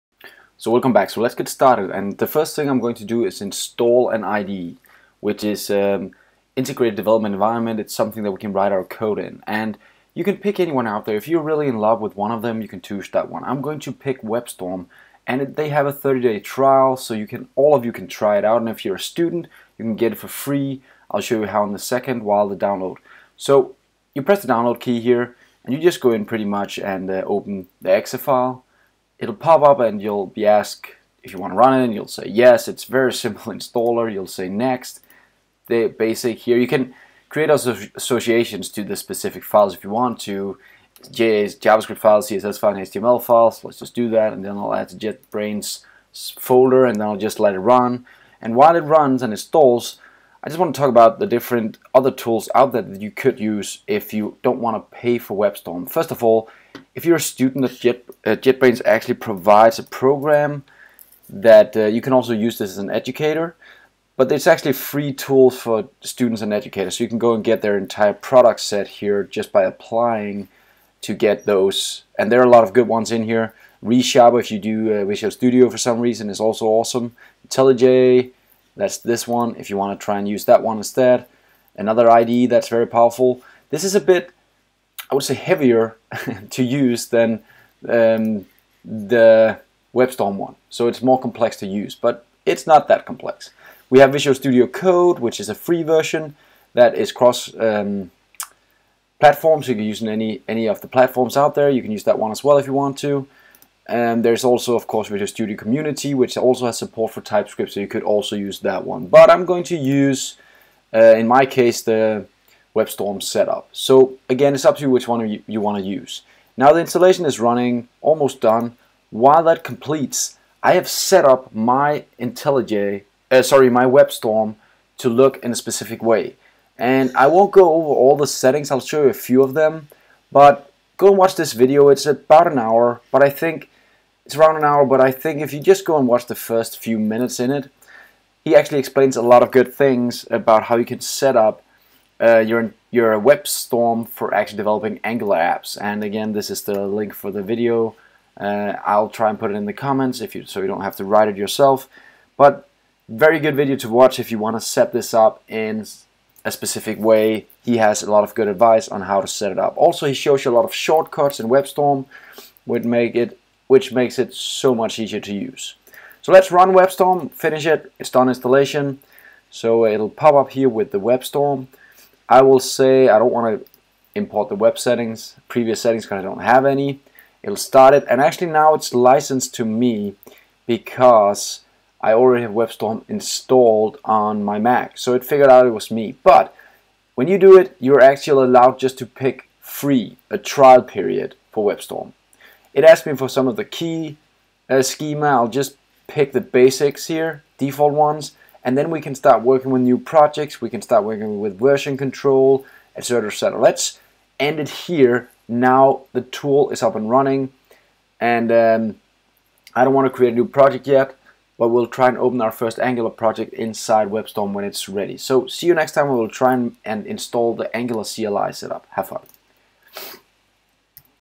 Windows WebStorm installation
This Series is about building a full-stack Web Applications using MEAN stack with Angular2 as the A in MEAN
We will build a course-planner application and will address most of the issues when developing a real online web application.

If you want more information of the MEN of MEAN and how we build a REST API in Mongo, ExpressJS and NodeJs go my series: